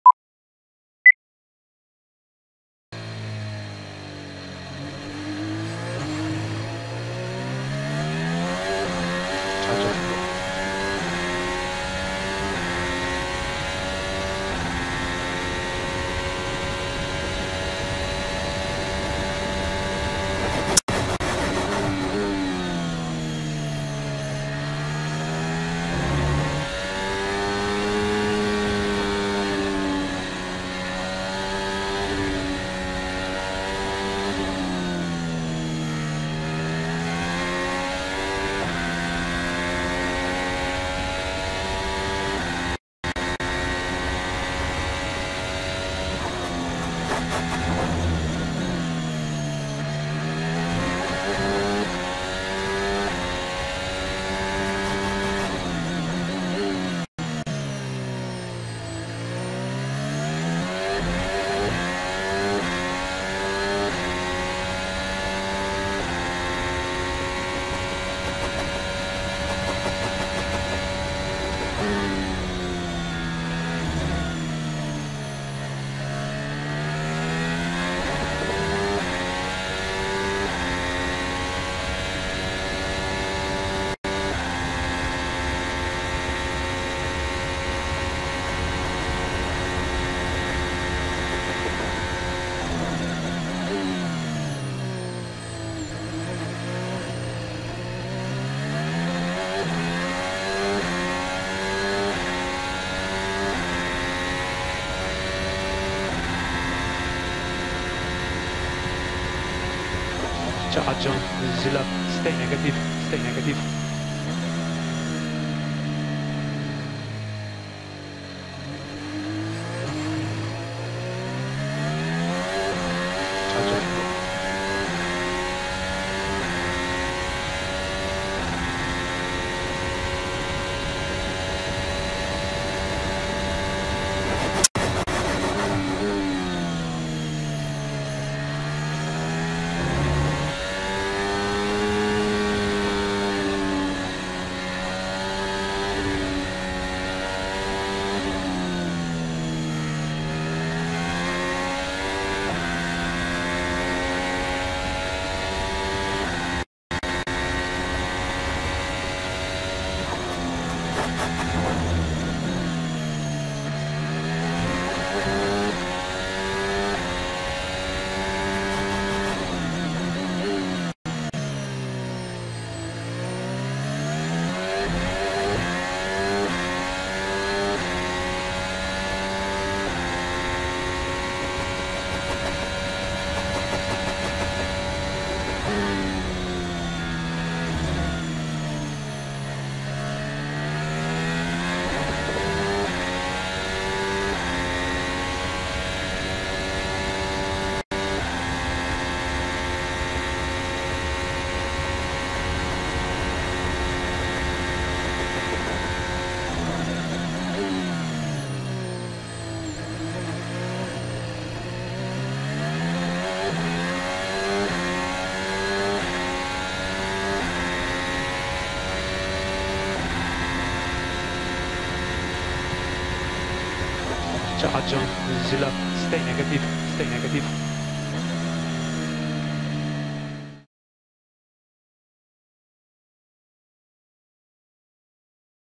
【F1予選】角田裕毅(Yuki Tsunoda) 日本GP(鈴鹿)オンボード | アルファタウリ
2022年のF1第18戦日本GP（鈴鹿）の予選が行われ、レッドブルのマックス・フェルスタッペンがポールポジションを獲得、フェラーリのシャルル・ルクレールは2番手、メルセデスのルイス・ハミルトンは6番手、アルファタウリ・ホンダの角田裕毅は13番手、ピエール・ガスリーは17番手の結果。
予選のオンボードとチームラジオ。